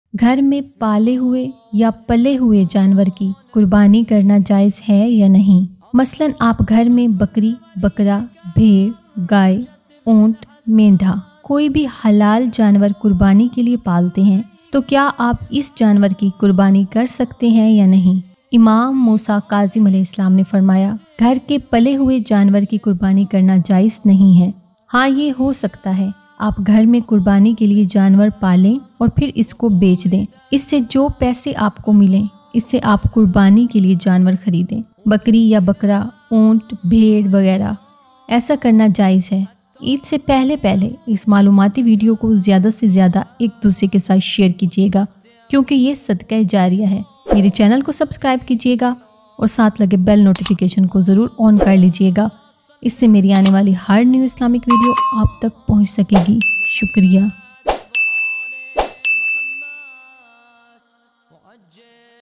Ghar Me Pally Howe Janwar Ki Qurbani Karna jaiz hy ? | Qurbani Ke Masail | Eid ul Adha 2023
Ghar Me Pally Howe Janwar Ki Qurbani Karna jaiz hy ? | Qurbani Ke Masail |    eid ul adha | Kis Janwar Ki Qurbani Nahi Hoti ?
 Es video main apky sath shiafiqih massla batay geya hy kia ghar mai palle howy animal ki Qurbani krna jaiz hy ya nahie Imam mosa kazim as ny frmaya jesa ky hum eid ul azha ky leye apny ghar main bakra (goat) ount (camel) bheer (sheep) ya koe halal janwar Eid ul adha py Allah ki raah (bargha ) main sacrifice ky leye pall lety hyn kia hum en janwaroon ki qurbani kar sakty hyn ya nahie ? apko detail ky sath btaya geya hy